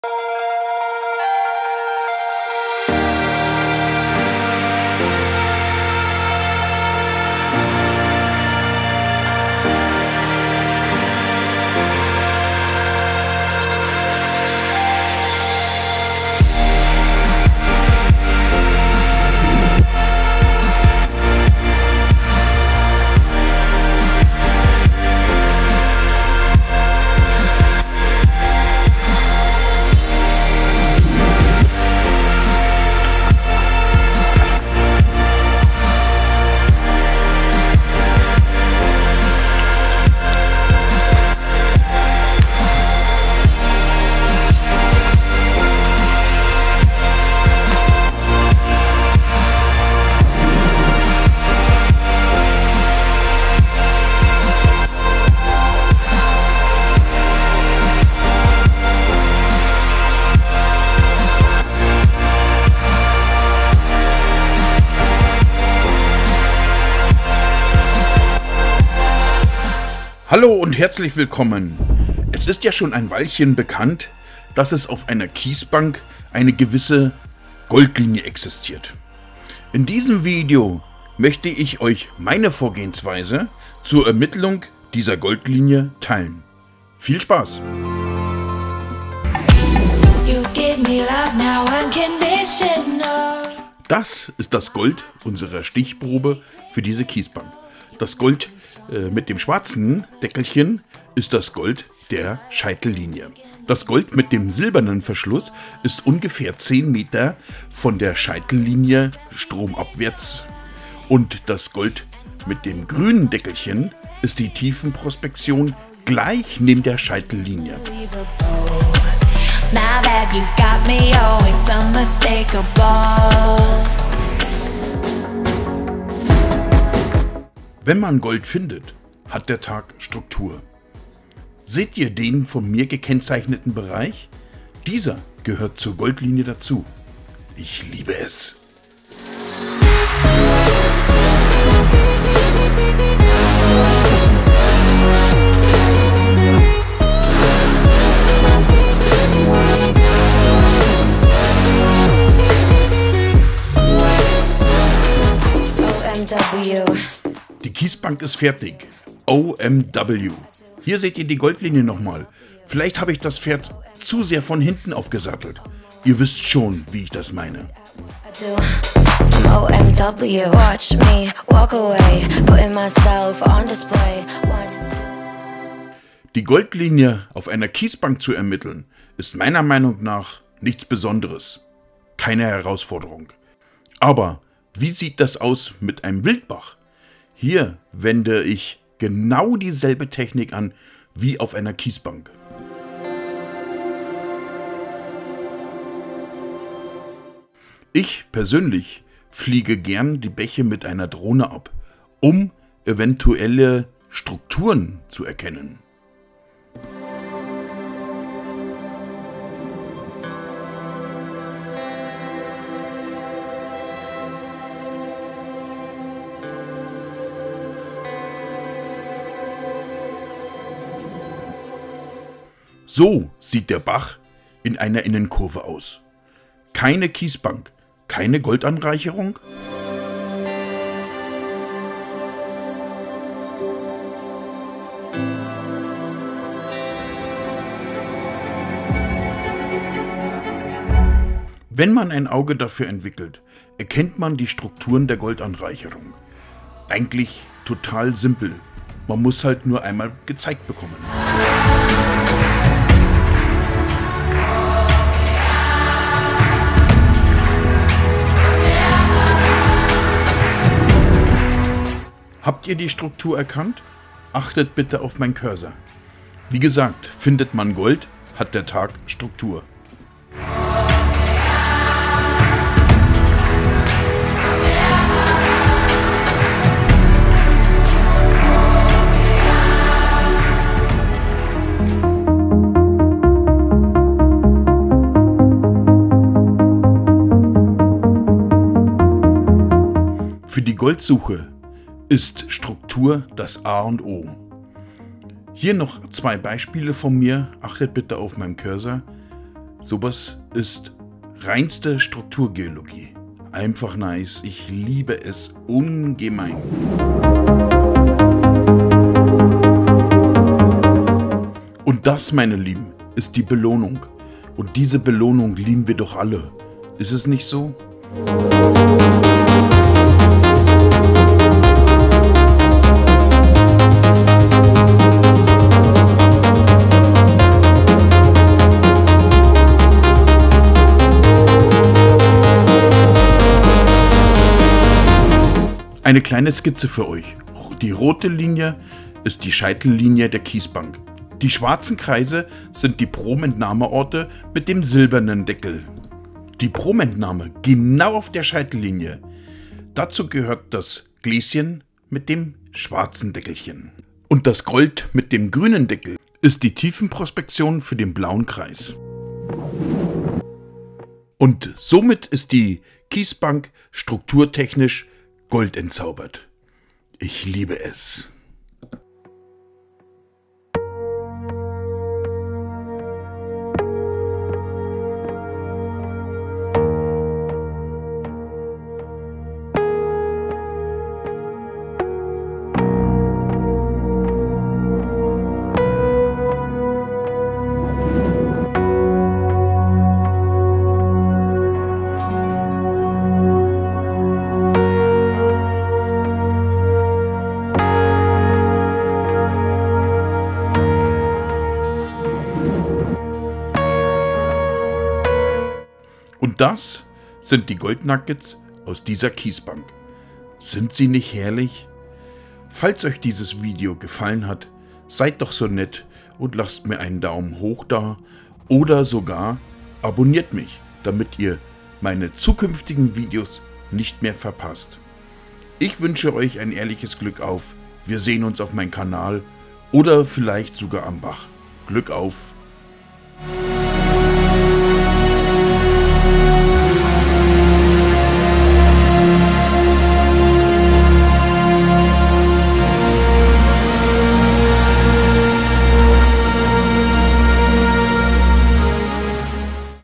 Die bayerischen Goldnuggets und wie finde ich die Goldlinie? [Abenteuer Goldwaschen]
Heute möchte ich euch einige Goldnuggets aus der bayerischen Mindel-Eiszeit zeigen und dazu meine Vorgehensweise zum finden der Goldlinie mit euch teilen. Die Goldlinie ist die Linie, in der sich das Gold am meisten anreichert.
Viel Spaß!

Ich bin TorstenMarx, bayerischer Goldschürfer seit über 10 Jahren und eins kann ich euch sagen, Goldwaschen ist das geilste Hobby der Welt!